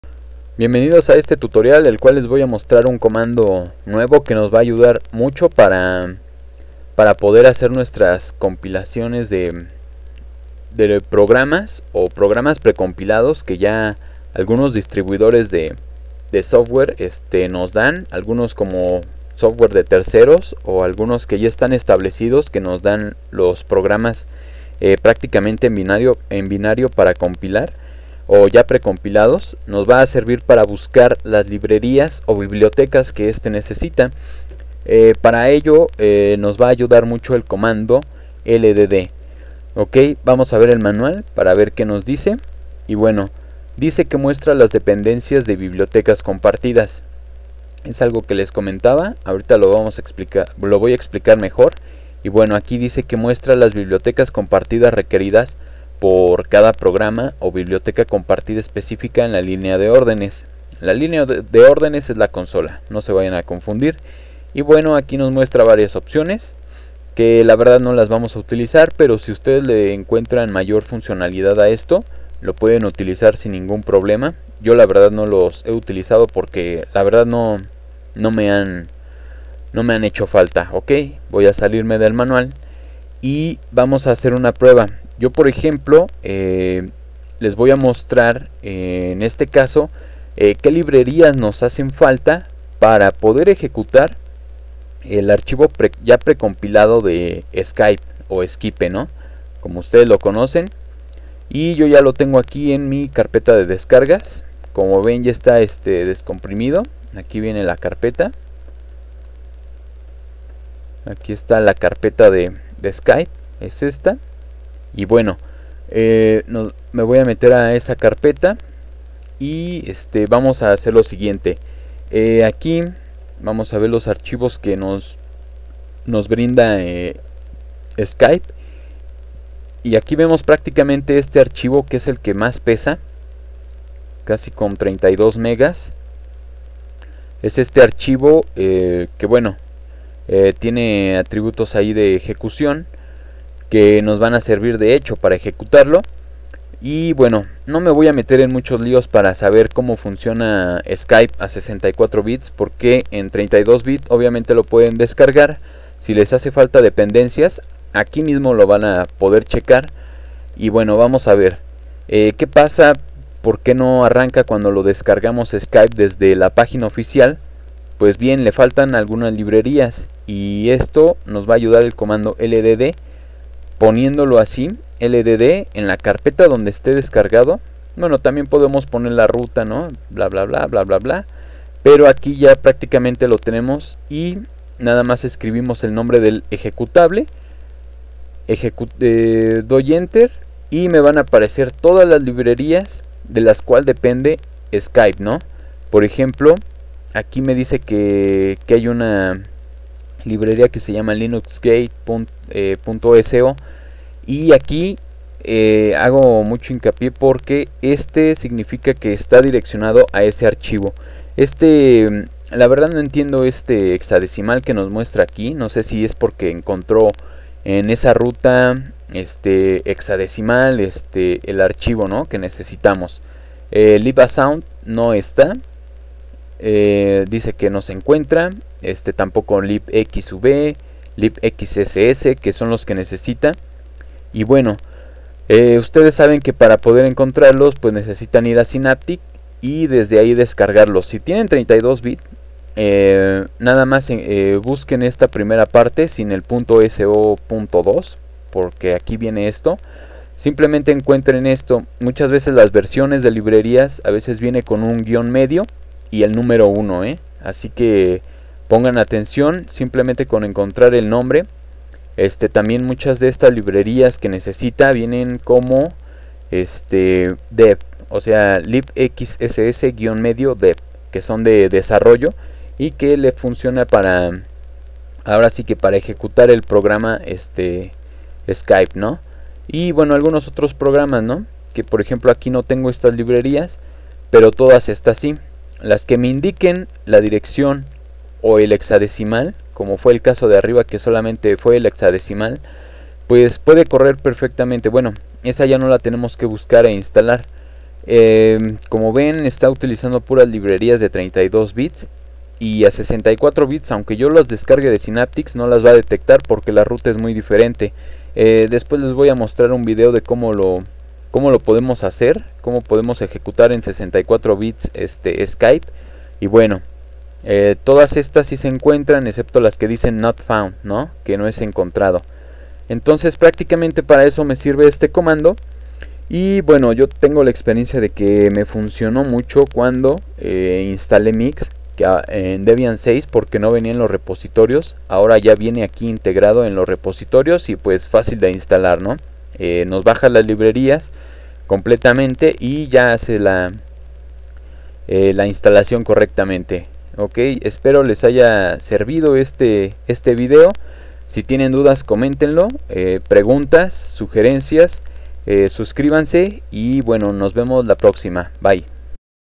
GNU/Linux desde la consola - Comando ldd
Comando que sirve para ver las dependencias de librerías de algunos programas que compilamos o nos lo dan precompilado listo para su uso, en este ejemplo explico como y que le falta de librerías a skype para su funcionamiento y esas librerías poder descargarlas de synaptic o conseguirlas.